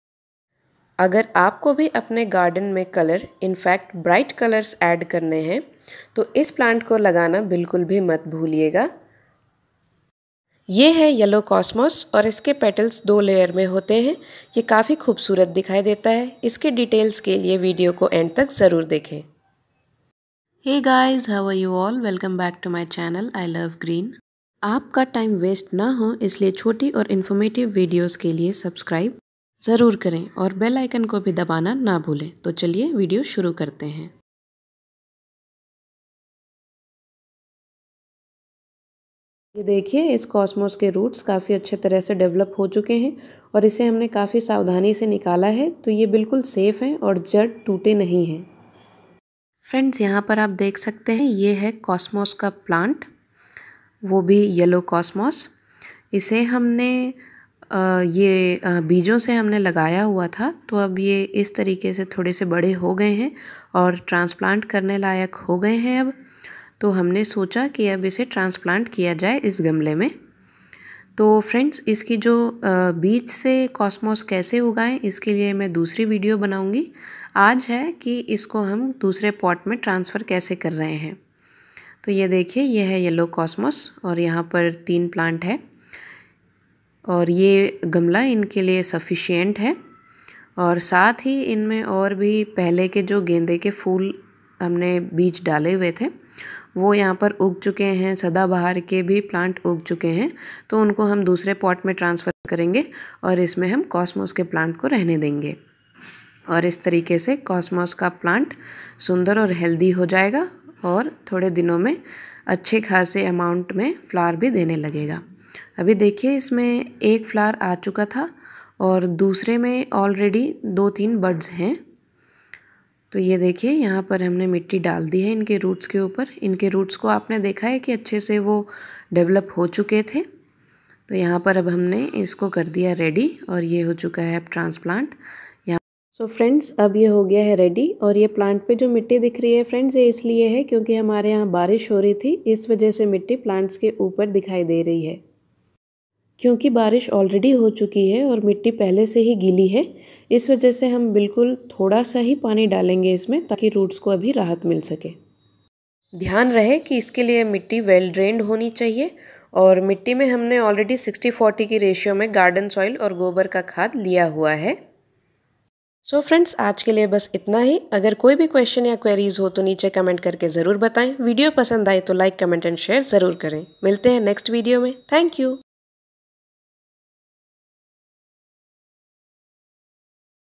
Grow yellow cosmos in summer/cosmos flower/cosmos plant care/best & beautiful summer flower
Namaste everyone 😊
This video is all about cosmos.
1. Grow yellow cosmos in summer
2. Cosmos flower
3. Cosmos plant care
4. Best & beautiful summer flower
In this video we are transplanting 3 yellow cosmos plant in the pot ... we have grown it from seeds then we safely took out the plant with its roots intact and now we are tranferring it to the new big pot so that it can get more space to grow happily ... cosmos comes in different colours as well this one is yellow with double layers of petals ... soil should be well drained and we have garden soil and cowdung in 60:40 ratio ... water it as per the requirement ... for other details watch the video till its end .
Thankyou 😊💐👍🕉